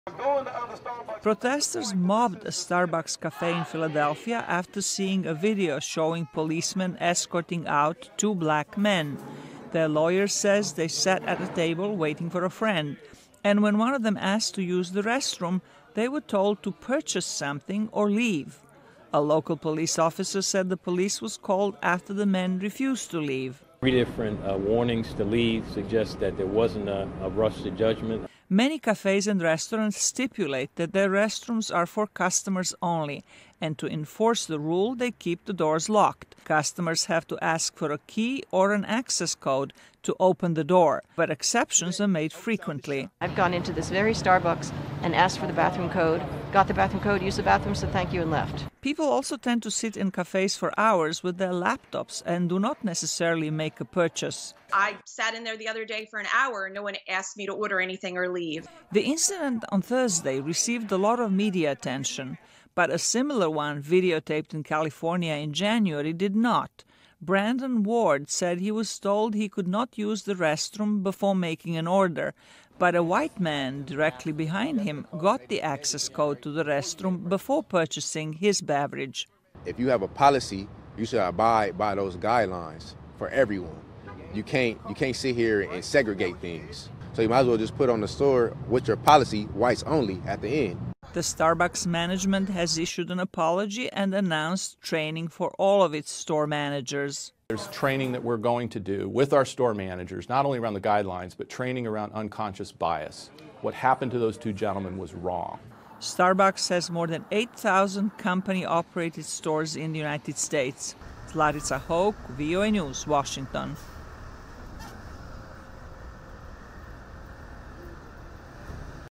Starbucks closes 8,000 stores for Racial Bias Training
Starbucks announced Tuesday that it will close thousands of its U.S. stores May 29 for staff training, following accusations that Starbucks employees discriminated against black people. Last week, a Starbucks store manager in Philadelphia called the police to remove two black men from the cafe.